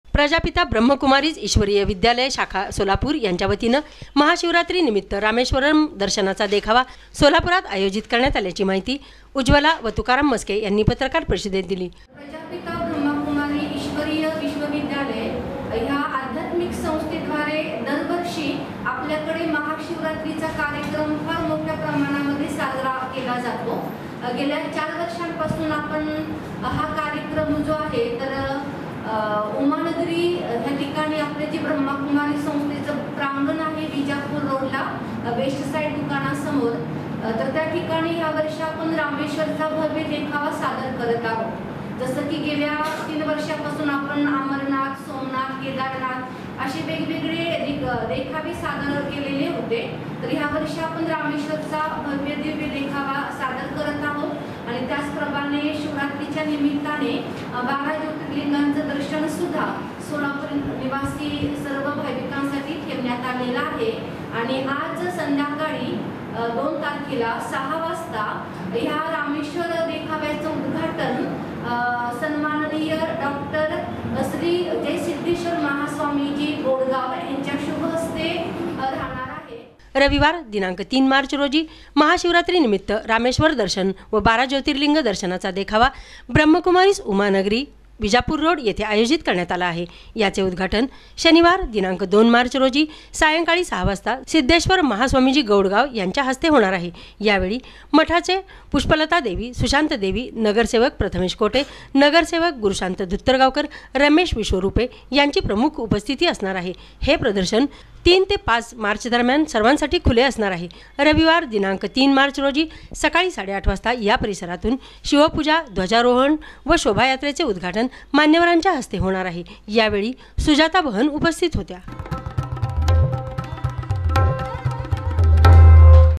सोलापुरात महाशिवरात्रीनिमित्त रामेश्वर दर्शनचे आयोजन
महाशिवरात्रीनिमित्त रामेश्वर दर्शन नाचे सोलापुरात आयोजन

प्रजापिता ब्रम्हकुमारीज ईश्वरीय विद्यालय शाखा सोलापूर यांच्या वतीने महाशिवरात्रीनिमित्त रामेश्वरम दर्शनाचे देखावा सोलापुरात आयोजित करण्यात आल्याची माहिती सुजाता व तुकाराम मस्के यांनी पत्रकार परिषदेत दिली.
रविवार 3 मार्च रोजी महाशिवरात महाशिवरात्रीनिमित्त रामेश्वर दर्शन व बारा ज्योतिर्लिंग दर्शनाचा देखावा ब्रह्मकुमारीस उमा नगरी विजापूर रोड येथे आयोजित करण्यात आला आहे या चे उद्घाटन शनिवार 2 मार्च रोजी सायंकाळी सहा वाजता सिद्धेश्वर महास्वामीजी गौडगावं यांच्या हस्ते होणार आहे. यावेळी मठाचे पुष्पलता देवी सुशांत देवी, नगरसेवक प्रथमेश कोठे नगरसेवक प्रशांत दुधगावकर रमेश विश्वरूपे  यांची प्रमुख उपस्थिती असणार आहे हे प्रदर्शन तीन ते पाच मार्च दरम्यान सर्वांसाठी खुले असणार आहे. रविवार 3 मार्च रोजी सकाळी साडेआठ वाजता या परिसरातून शिवपूजा ध्वजारोहन व शोभायात्रेचे उद्घाटन मान्यवरांच्या हस्ते करण्यात येणार आहे यावेळी सुजाता बहन उपस्थित होते